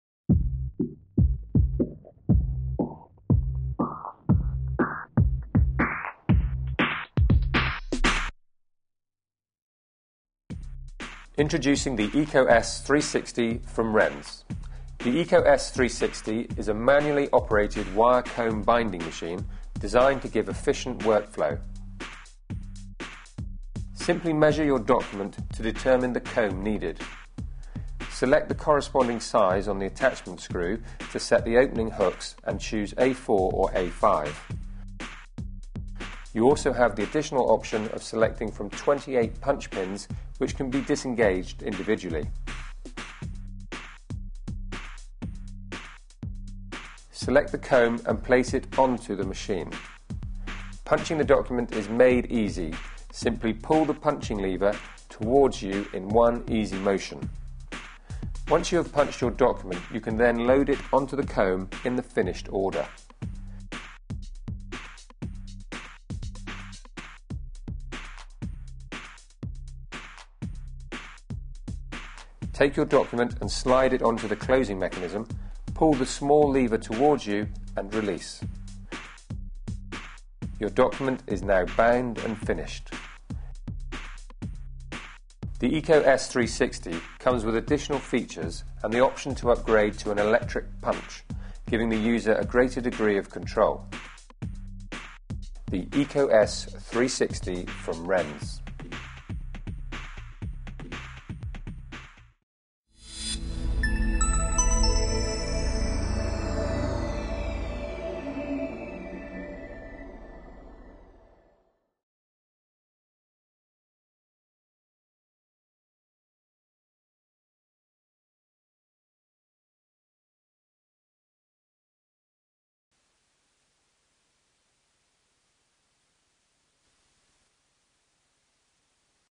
RENZ eco S 360 Drahtbindegerät mit 2:1 Teilung
Die "RENZ Eco S Drahtbindemaschine" hat eine Stanzleistung für ca. 30 Blatt und besitzt eine Bindestärke von bis zu 320 Blatt. Sie verarbeitet Draht bis 38mm und alle Formate bis A3. Alle 28 Messer sind abschaltbar. Die Maschine ist jederzeit mit einem
Elektroantrieb nachrüstbar.